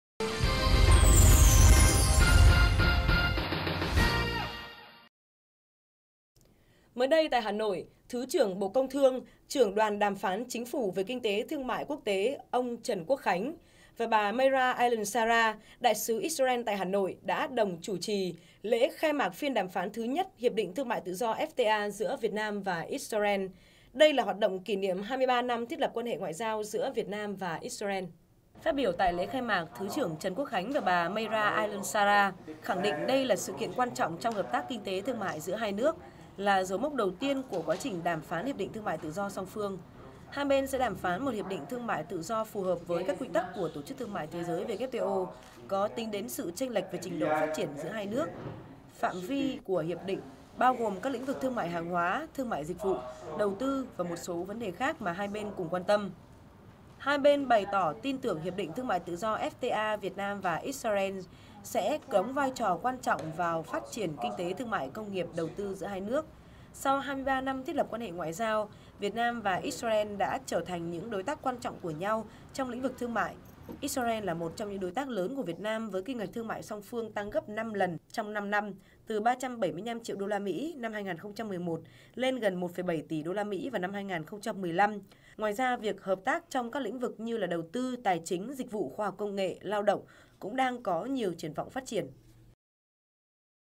Khai mạc Phiên đàm phán Hiệp định thương mại tự do giữa Việt Nam & I-xra-en
Tại Hà Nội, thứ trưởng Bộ Công Thương, trưởng đoàn đàm phán Chính phủ về kinh tế và thương mại quốc tế - ông Trần Quốc Khánh và bà Meirav Elion Shahar, đại sứ I-xra-en tại Việt Nam đã đồng chủ trì Lễ khai mạc Phiên đàm phán thứ nhất Hiệp định thương mại tự do (FTA) giữa Việt Nam và I-xra-en. Đây là hoạt động kỷ niệm 23 năm thiết lập quan hệ ngoại giao giữa Việt Nam và I-xra-en...